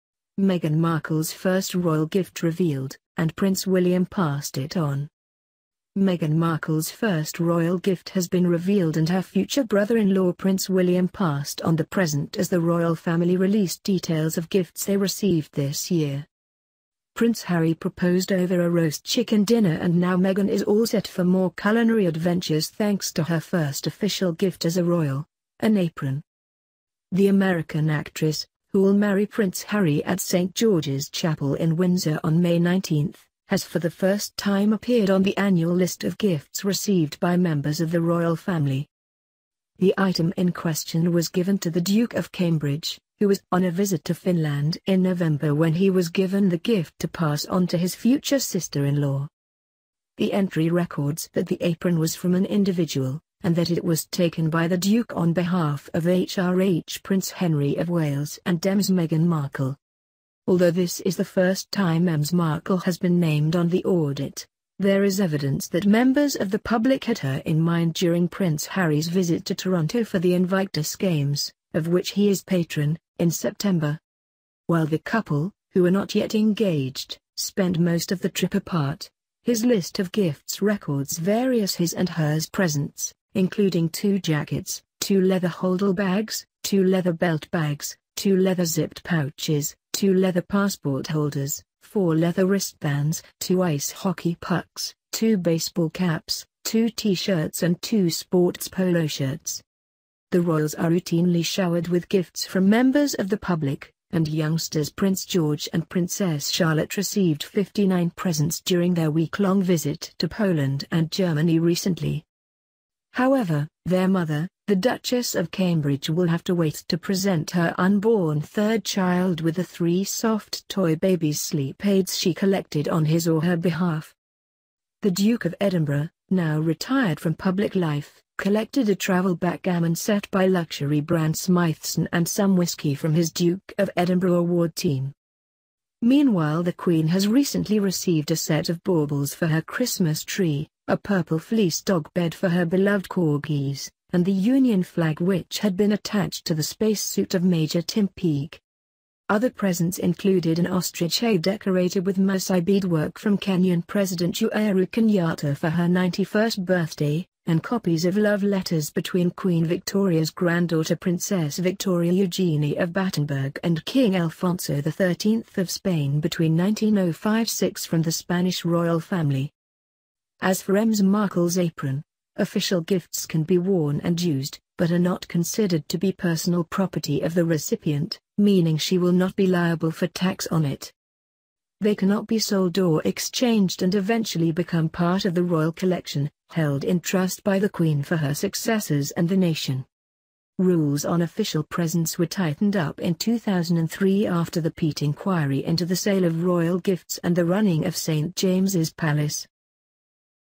Meghan Markle's first royal gift revealed - and Prince William passed it on
MEGHAN Markle's first royal gift has been revealed and her future brother-in-law Prince William passed on the present as the Royal Family released details of gifts they received this year.

Prince Harry proposed over a roast chicken dinner and now Meghan is all set for more culinary adventures thanks to her first official gift as a royal – an apron.

The American actress, who will marry Prince Harry at St George's Chapel in Windsor on May 19, has for the first time appeared on the annual list of gifts received by members of the Royal Family.

The item in question was given to the Duke of Cambridge, who was on a visit to Finland in November when he was given the gift to pass on to his future sister-in-law.

The entry records that the apron was from an “individual”, and that it was taken by the Duke “on behalf of HRH Prince Henry of Wales and Ms Meghan Markle”.

Although this is the first time Ms Markle has been named on the audit, there is evidence that members of the public had her in mind during Prince Harry’s visit to Toronto for the Invictus Games, of which he is patron, in September.

While the couple, who were not yet engaged, spent most of the trip apart, his list of gifts records various his-and-hers presents, including “two jackets, two leather holdall bags, two leather belt-bags, two leather zipped pouches, two leather passport holders, four leather wristbands, two ice hockey pucks, two baseball caps, two t-shirts and two sports polo shirts”.

The Royals are routinely showered with gifts from members of the public, and youngsters Prince George and Princess Charlotte received 59 presents during their week-long visit to Poland and Germany recently.

However, their mother, the Duchess of Cambridge will have to wait to present her unborn third child with the three “soft toy baby's sleep aids” she collected on his or her behalf.

The Duke of Edinburgh, now retired from public life, collected a travel backgammon set by luxury brand Smythson and some whisky from his Duke of Edinburgh Award team.

Meanwhile the Queen has recently received a set of baubles for her Christmas tree, a purple fleece dog bed for her beloved corgis, and the Union Flag which had been attached to the spacesuit of Major Tim Peake.

Other presents included an ostrich egg decorated with Maasai beadwork from Kenyan president Uhuru Kenyatta for her 91st birthday, and copies of love letters between Queen Victoria’s granddaughter Princess Victoria Eugenie of Battenburg and King Alfonso XIII of Spain between 1905-6 from the Spanish Royal Family.

As for Ms Markle’s apron, official gifts can be worn and used, but are not considered to be personal property of the recipient, meaning she will not be liable for tax on it.

They cannot be sold or exchanged and eventually become part of the Royal Collection, held in trust by the Queen for her successors and the nation.

Rules on official presents were tightened up in 2003 after the Peat inquiry into the sale of royal gifts and the running of St James's Palace.

Meghan Markle, with Prince Harry in the background, during a walkabout at Cardiff Castle,
Prince Harry and Meghan Markle, pictured days after announcing their engagment,
Prince George and Princess Charlotte collected 59 gifts during last year' trip to Germany and Poland,
Queen Elizabeth II chats to New Zealand PM John Key – with a corgi in close attendence,